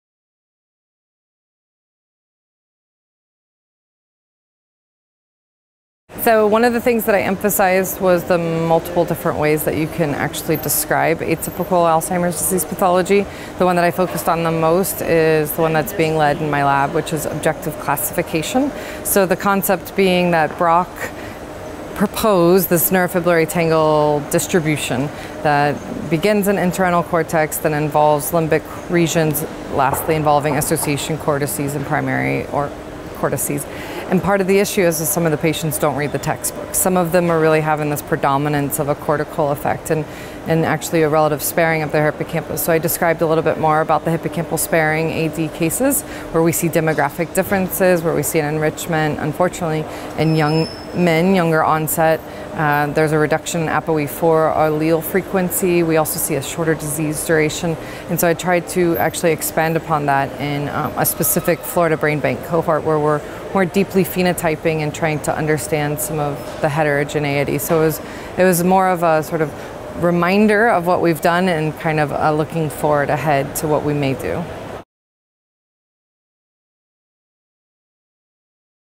How can we describe atypical Alzheimer’s pathology?
The heterogeneous nature of Alzheimer’s disease (AD) can make it difficult to understand the disorder’s pathology. Melissa Murray, PhD from the Mayo Clinic, Jacksonville, FL talks to us about her presentation at the 2017 Alzheimer’s Association International Conference (AAIC) in London, UK, which emphasized the multiple ways which can be used to describe atypical AD pathology. Dr Murray also describes some of the different pathologies of AD, and speaks about a study involving a brain bank in Florida.